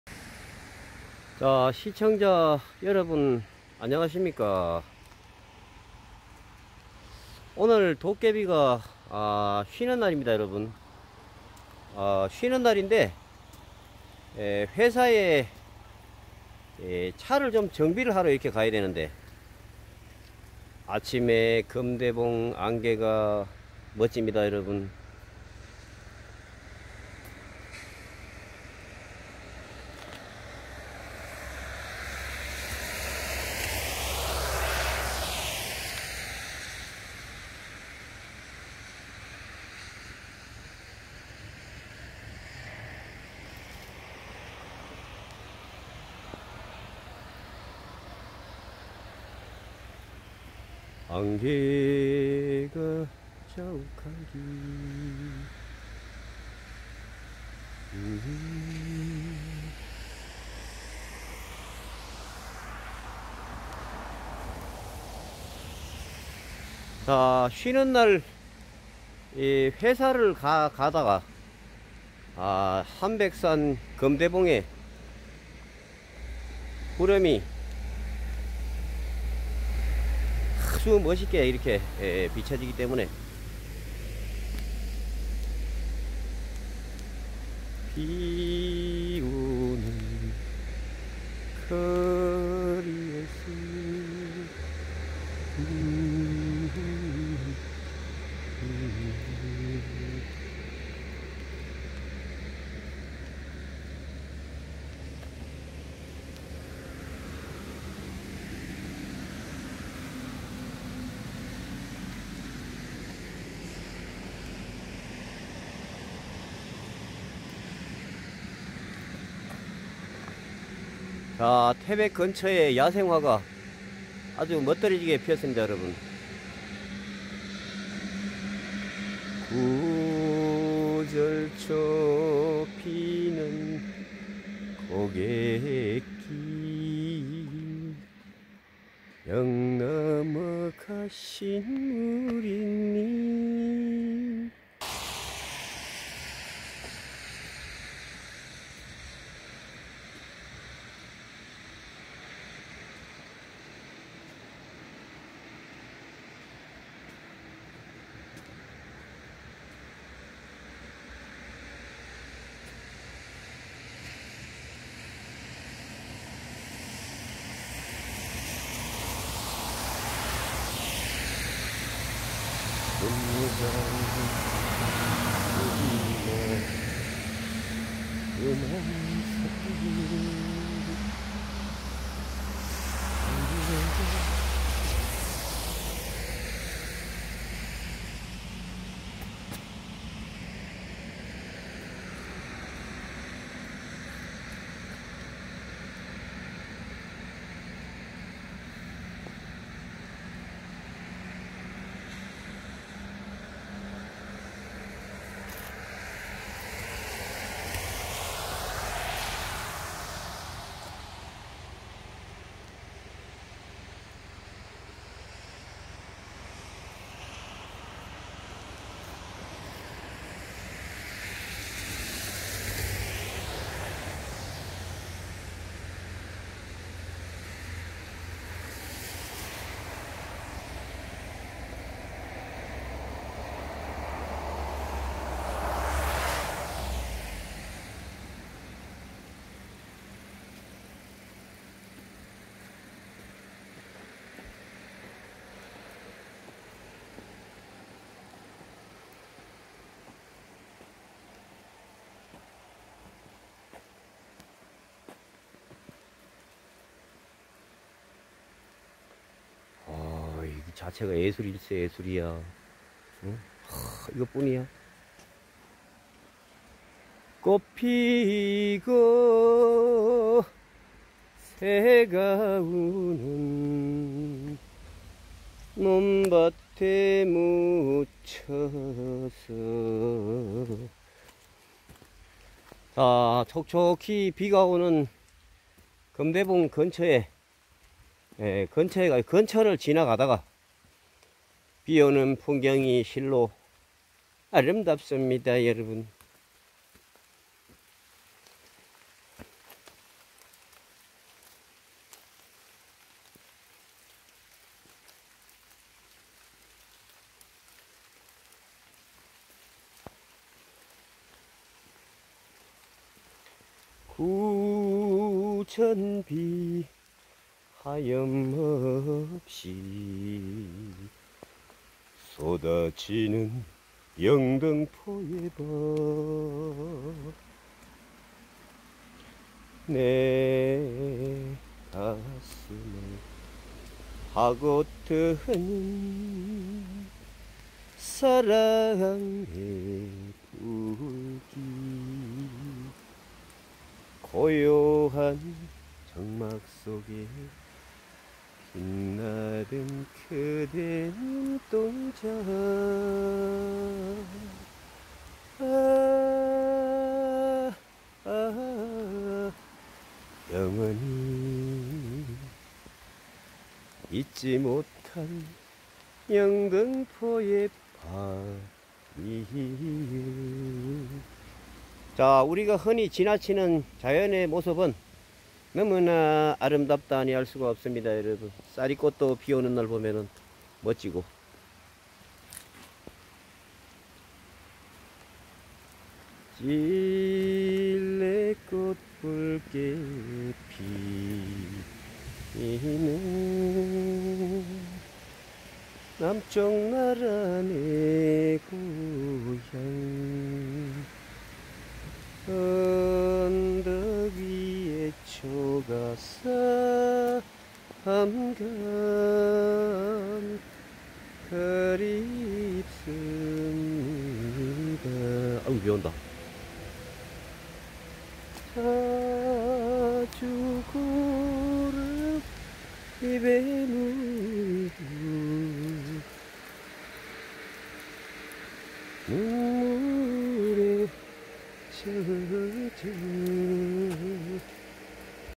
도깨비 쉬는날 회사를 가던길 함백산 금대봉 구름이 산을 휘감고 초가을ㆍ
함백산 도깨비 쉬는날 회사를 가던길 높은산 금대봉의
초가을 풍경이 너무 좋더군 그래서 화면에 담았는데ᆢ